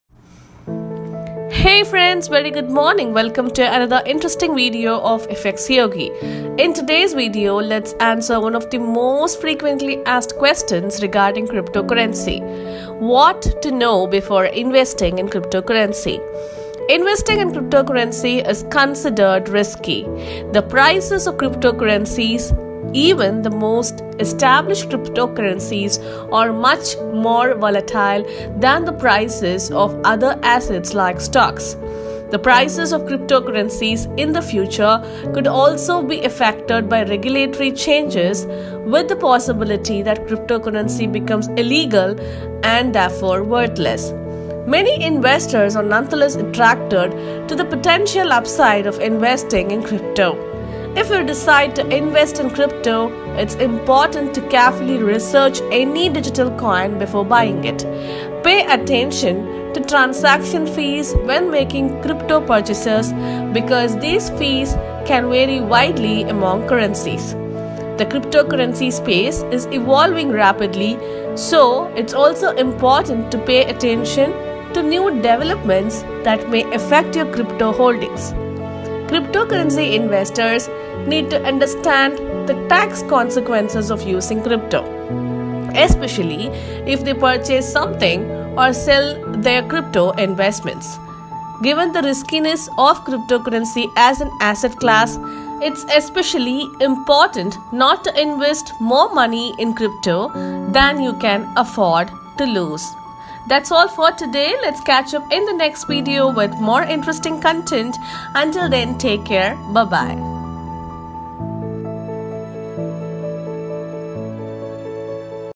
What to Know Before Investing in Cryptocurrency
Investing in cryptocurrency is considered risky. The prices of cryptocurrencies, even the most established cryptocurrencies, are much more volatile than the prices of other assets like stocks. The prices of cryptocurrencies in the future could also be affected by regulatory changes, with the possibility that cryptocurrency becomes illegal and therefore worthless.
Many investors are nonetheless attracted to the potential upside of investing in crypto. If you decide to invest in cryptocurrency, it's important to carefully research any digital coin before buying it. Pay attention to transaction fees when making crypto purchases because these fees can vary widely among currencies.
The cryptocurrency space is evolving rapidly, so it's also important to pay attention to new developments that may affect your crypto holdings. Cryptocurrency investors need to understand the tax consequences of using crypto, especially if they purchase something or sell their crypto investments.
Given the riskiness of cryptocurrency as an asset class, it's especially important not to invest more money in crypto than you can afford to lose.

Why FxYogi?
We provide managed Forex trading services to a wide spectrum of clients from various geographical locations.